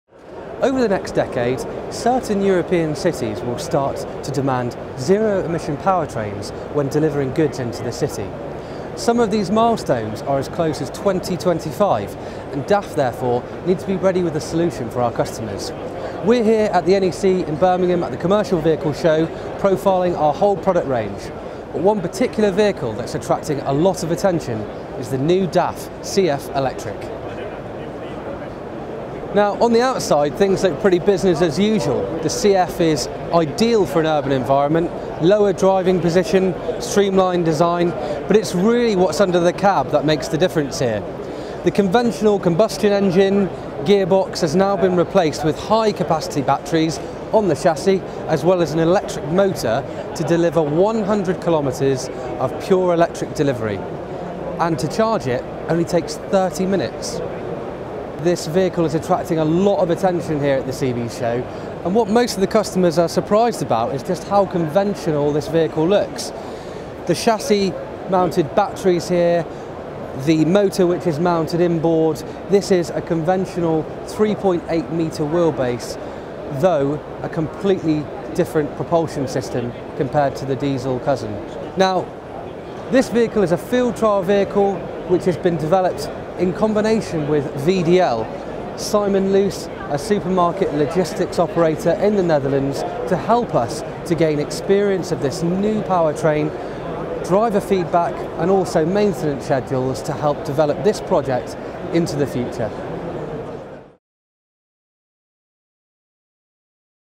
Electric DAF CF Truck Walk-Around | CV Show 2019
James Tuner shows us round the New CF Electric truck on the DAF stand at the Commercial Vehicle show.